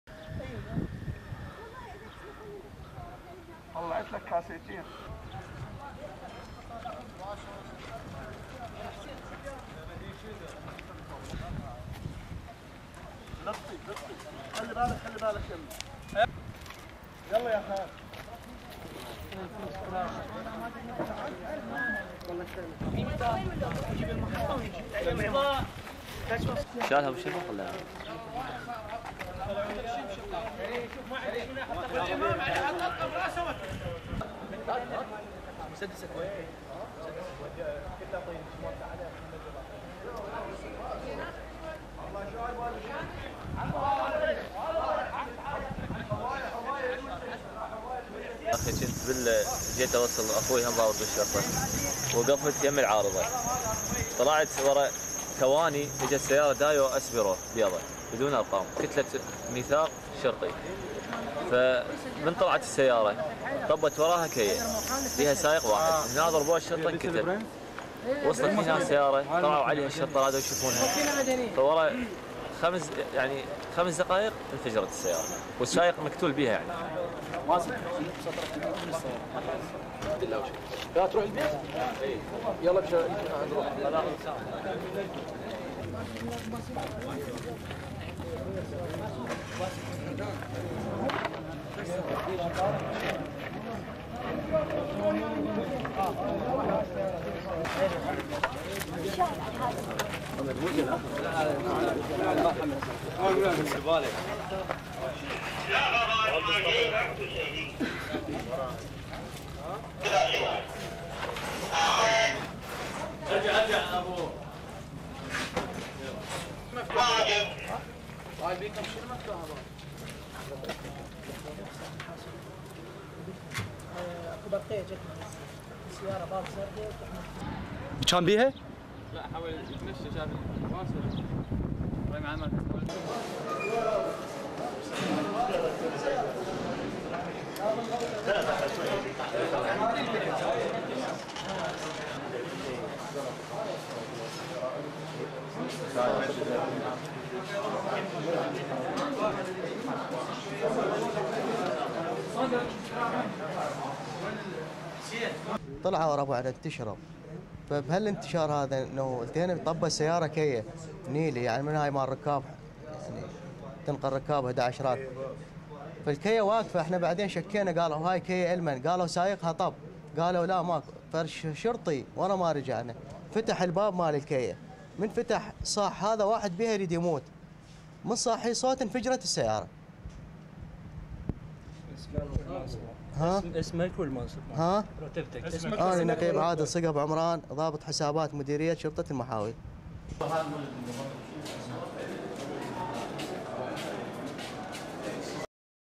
Militants attack police station, killing five
1. Various shots of crater from blast
2. Scorched wall
3. People mulling around
4. Various shots of damaged vehicles
5. SOUNDBITE: (Arabic) Mazen Badr, Eyewitness:
"I was giving my brother a lift and I had driven to to the checkpoint. A few seconds later the car drove behind (us). The insurgents inside killed the policeman at the checkpoint. They drove away and a minibus came up behind. There was only the driver in it. The police rushed towards it. After a few minutes the minibus exploded."
6. Damaged building
7. Debris
8. Various shots of shoes
9. Destroyed motorcycle
10. Damaged interior
11.  Chair with "Iraqi Police" armband
12. "Iraqi Police" armband
13. Damaged stairs
14. Wide shot of scene
15. Various shots of injured in Al-Mahaweel Hospital
16. SOUNDBITE: (Arabic) Captain Adil Sugab, Iraqi Police:
"One of our officers guarding the station was killed. We then deployed. A minibus was parked near us. We were suspicious and we wondered where the driver was. One of our officers opened the door to the minibus. He shouted to us that there was a dying man inside. As he shouted the minibus exploded."
17. People in hospital corridor

STORYLINE:

In a town south of Baghdad on Thursday, a pair of attackers dressed in police uniforms opened fire at guards outside a police station while a third barrelled toward the station in a vehicle filled with explosives and blew up.

The attack killed five people and wounded 27 others, the Interior Ministry said.

The bombing, in the town of Mahawil, 85 kilometres (50 miles) south of Baghdad, appeared to signal a new level of coordination in insurgent attacks on police, whom they see as collaborators with US forces.

The blast damaged the gate of the station and a dozen nearby cars - and left a five-meter-wide crater in the ground.

Captain Adil Sugab, of the Iraqi police force, said: "A minibus was parked near us. We were suspicious and we wondered where the driver was. One of our officers opened the door to the minibus. He shouted to us that there was a dying man inside. As he shouted the minibus exploded."

The two gunmen escaped, said Saab Kamahi, an Interior Ministry spokesman.

Police Lieutenant Colonel Arak al-Husseini said the assailants were dressed in police uniform.